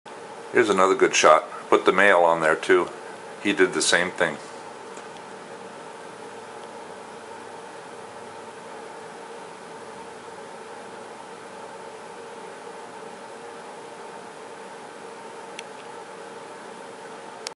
Rearing Monarch Butterflies - Clip 17 of 20 - More bananas. Yum!
Camera: Sony DSC-T900. Visit my web site for more videos of interest to insect collectors